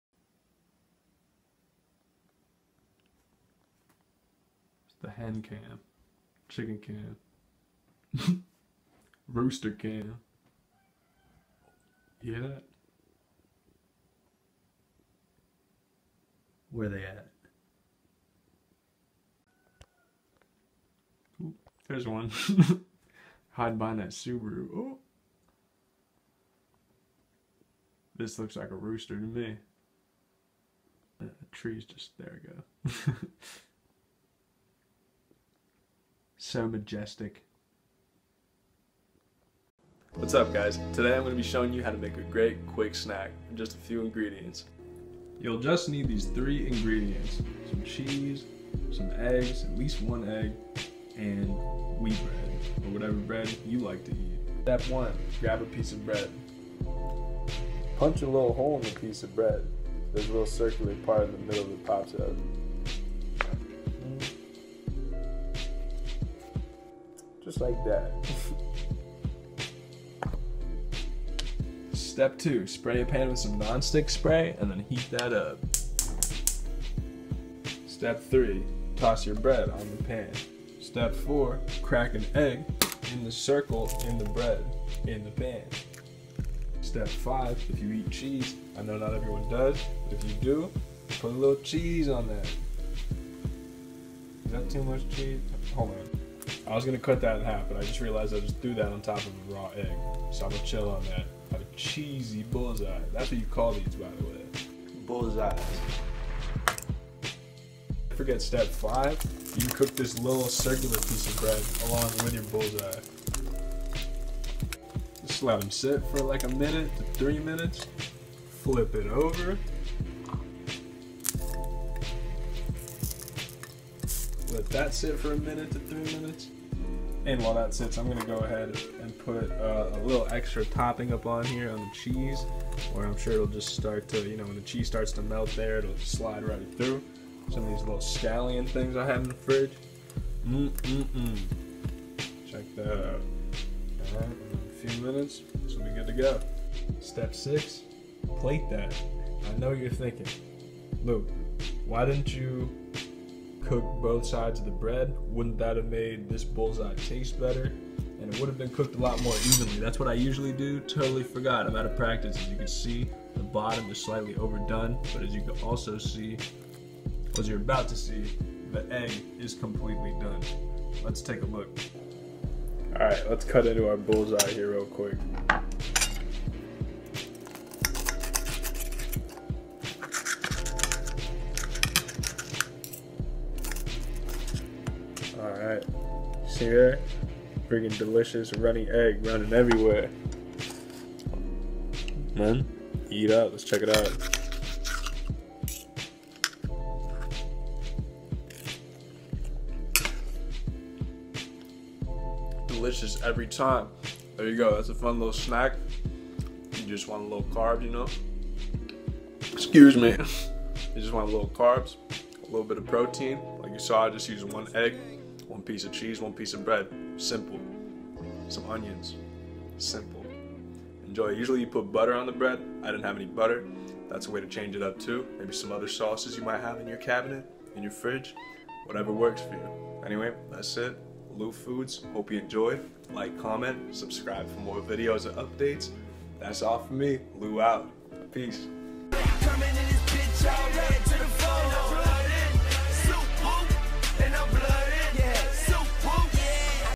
Bullseyes
A nice little snack that doesn't involve too many ingredients.  Enjoy!